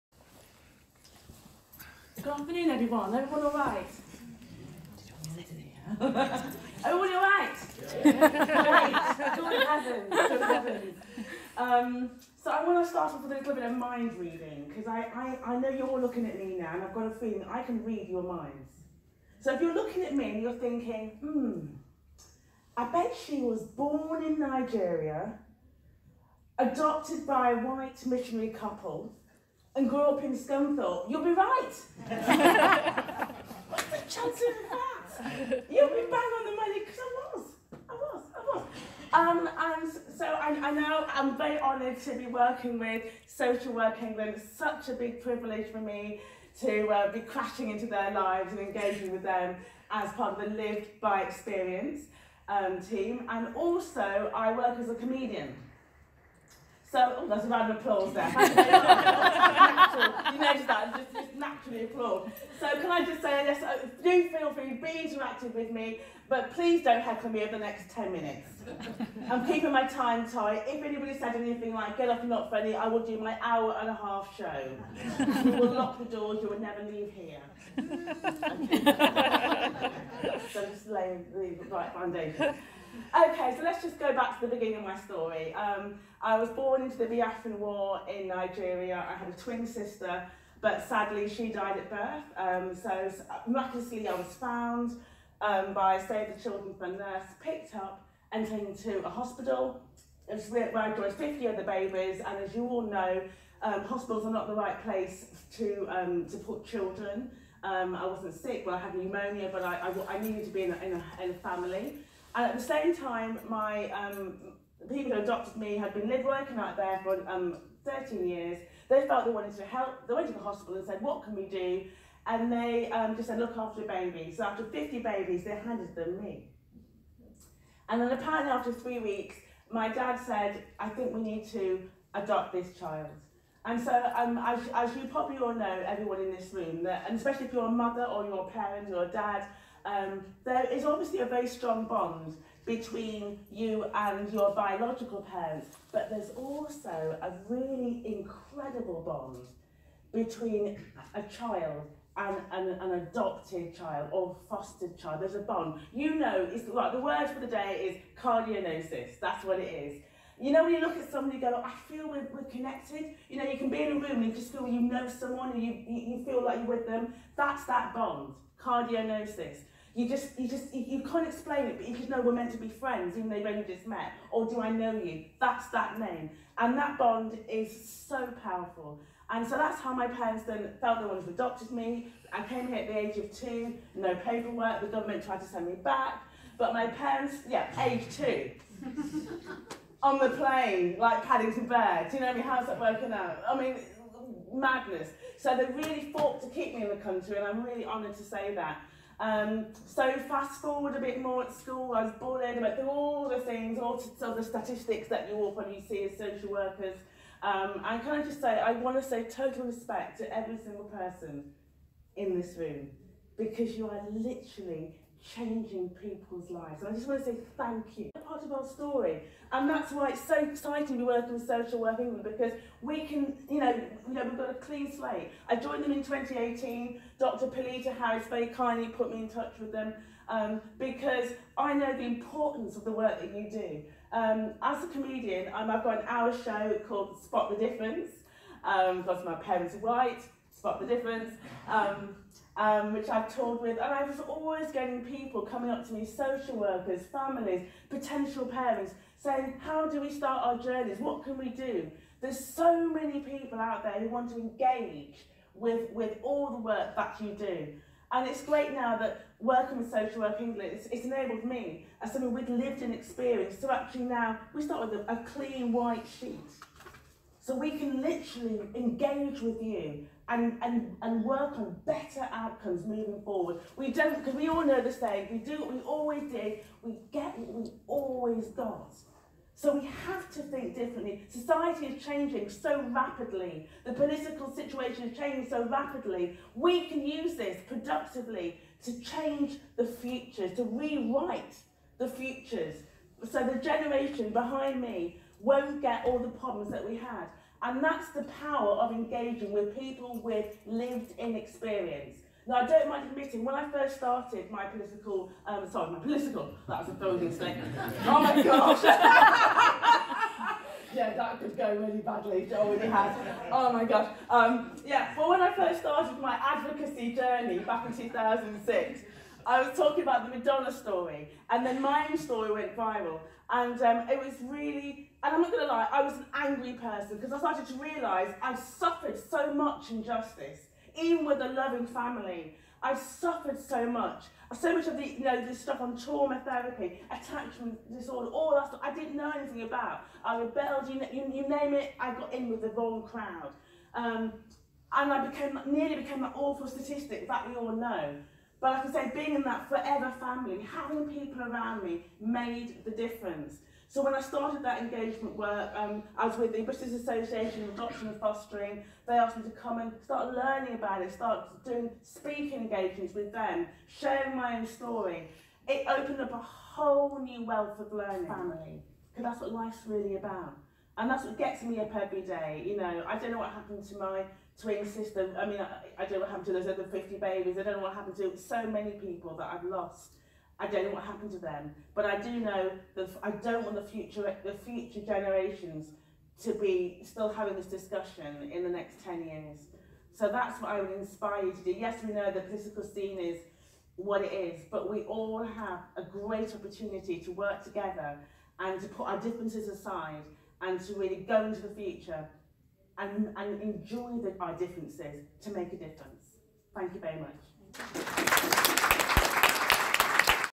Speaking at National Children and Adult Services Conference 2019
I was honoured to speak at the National Children and Adult Services Conference 2019 with Social Work England representing Experts with Experience regarding my adoption and social work knowledge.  It was an exciting day in Bournemouth at the International conference centre on 20th November. Please do listen back to 7 minutes of my 12 minute presentation.

*for more of my comedy adoption speaking work/shows/engagement please visit Joycarter.co.uk

Sarah Blackmore, Director of Standards for Social Work England opened our section, followed by Phillipa Geddes, Regional Engagement Lead and chaired by Suzanne Joyner  Director of Children and Adult Services, Darlington Borough Council.
Great day